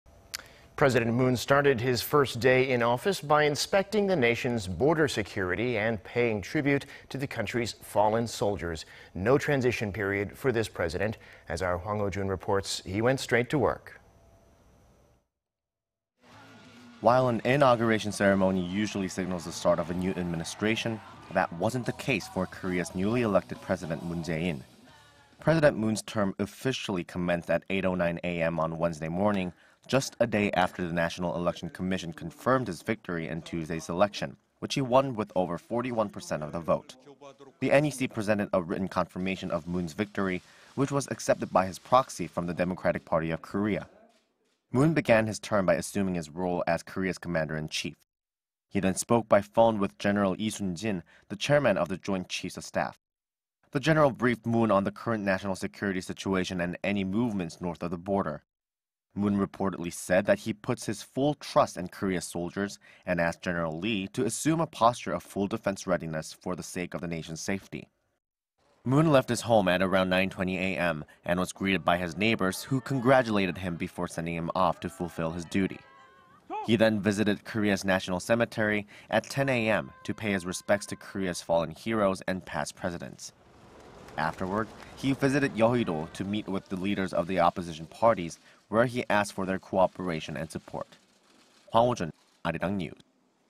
Moon Jae-in begins first day of five-year presidency
문재인 대통령 임기 개시

  President Moon started his first day in office... by inspecting the nation's border security and paying tribute to the country's fallen soldiers.
No transition period for this President.
As our Hwang Ho-jun reports, he went straight to work.
  While an inauguration ceremony usually signals the start of a new administration,... that wasn't the case for Korea's newly elected president,... Moon Jae-in.
President Moon's term officially commenced at 8:09 a.m. on Wednesday morning, just a day after the National Election Commission confirmed his victory in Tuesday's election,... which he won with over 41-percent of the vote.
The NEC presented a written confirmation of Moon's victory, which was accepted by his proxy from the Democratic Party of Korea.
  Moon began his term by assuming his role as Korea's commander in chief.
He then spoke by phone with General Lee Sun-jin, the chairman of the Joint Chiefs of Staff.
The general briefed Moon on the current national security situation and any movements north of the border.
Moon reportedly said that he puts his full trust in Korea's soldiers and asked General Lee to assume a posture of full defense readiness for the sake of the nation's safety.

  Moon left his home at around 9:20 a.m., and was greeted by his neighbors, who congratulated him before sending him off to fulfill his duty.
He then visited Korea's National Cemetery at 10 a.m. to pay his respects to Korea's fallen heroes and past presidents.
  Afterward he visited Yeoido to meet with the leaders of the opposition parties, where he asked for their cooperation and support.